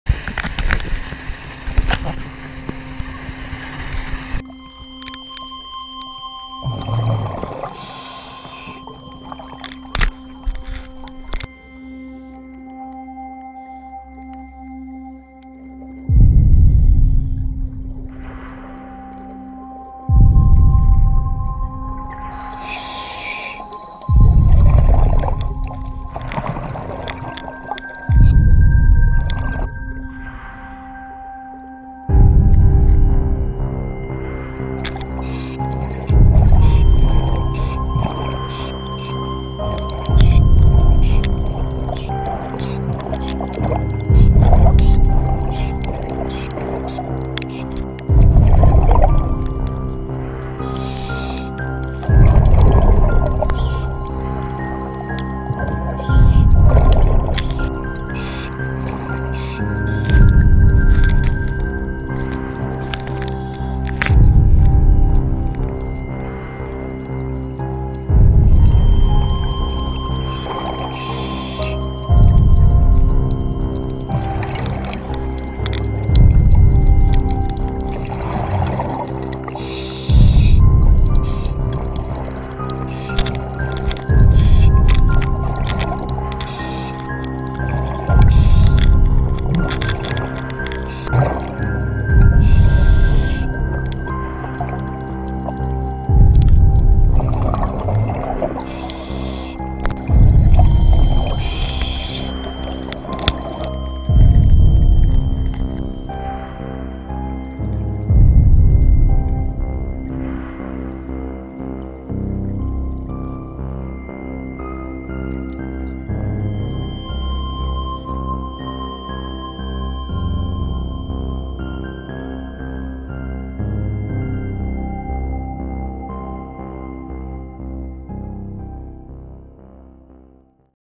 Tug Boat
Diving the Tug Boat in RAK with PADI Tec Trimix Instructor trainer and   Course Director - Imad Khashfeh